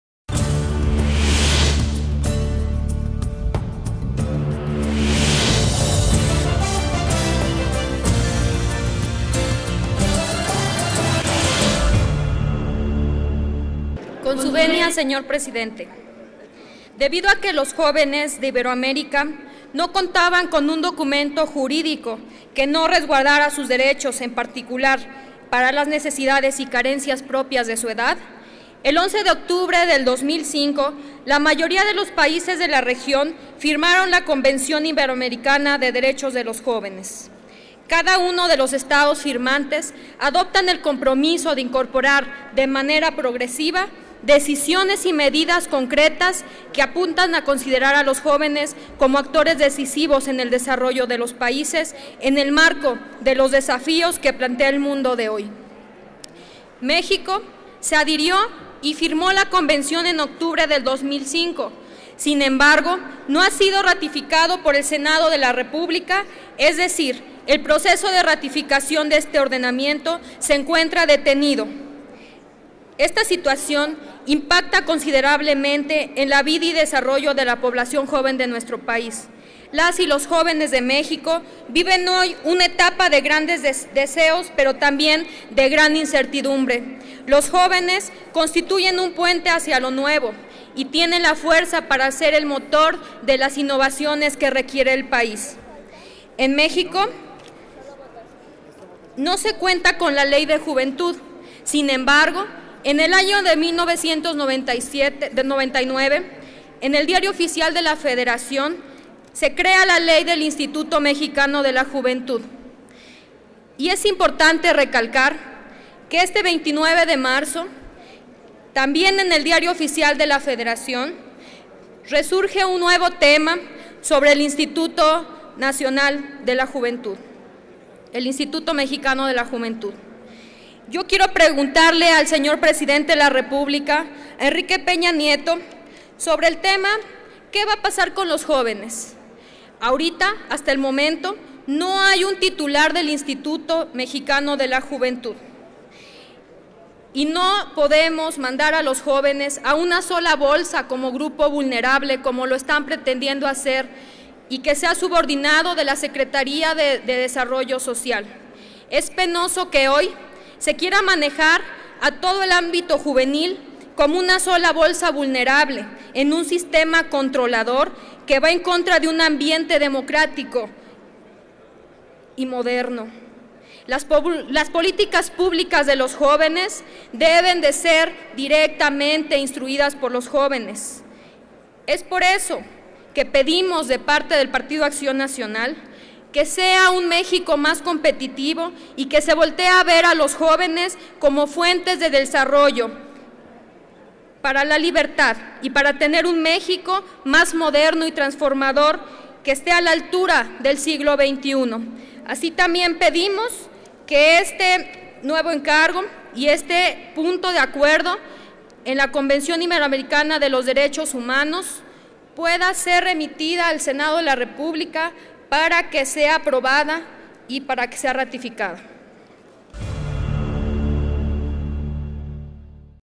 Dip. María Teresa Jiménez (PAN) - Convención Iberoamericana de los Derechos de los Jóvenes
La Dip. María Teresa Jiménez Esquivel (PAN) recuerda que los jóvenes de Iberoamérica al no encontrar oportunidades impulsaron la firma de la Convención Iberoamericana de los Derechos de los Jóvenes, el 11 de octubre de 2005, a fin de considerarlos como actores decisivos en la elaboración de políticas públicas. Asimismo, sostiene que los recursos orientados a dicho sector deben ser ejercidos por jóvenes.